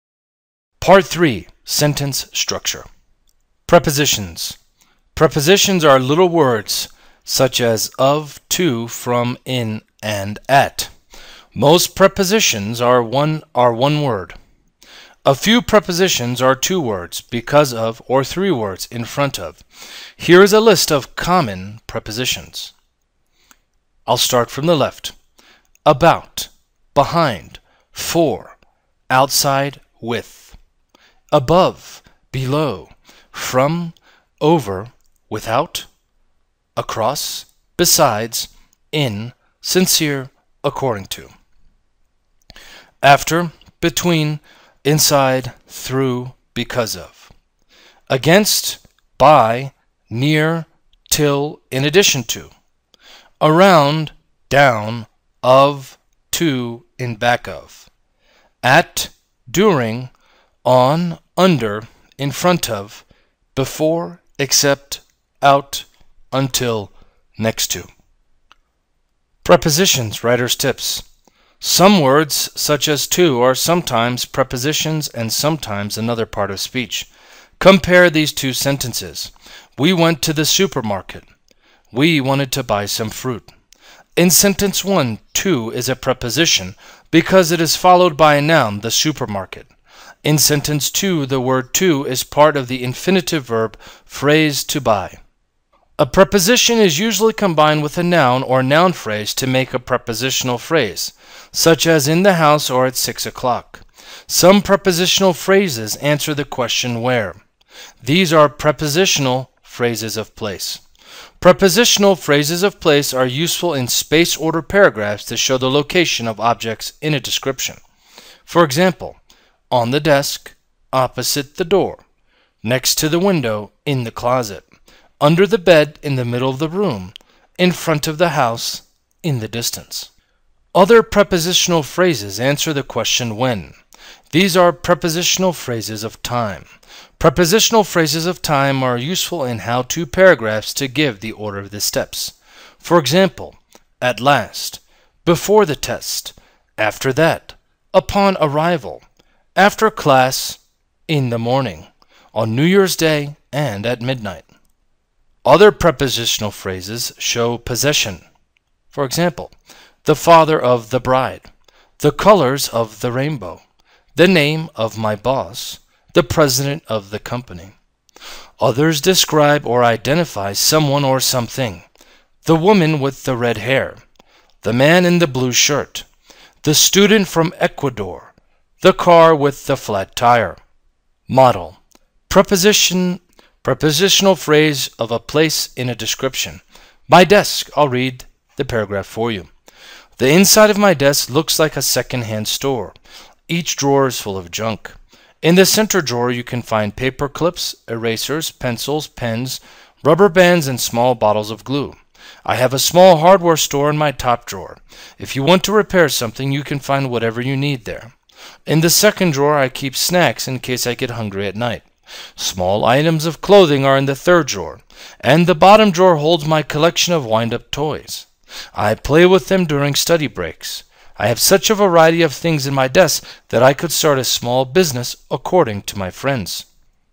Technical Report Writing | U4-L7 | Preposition and Prepositional Phrase
Model Paragraph: Prepositional phrase of space in a description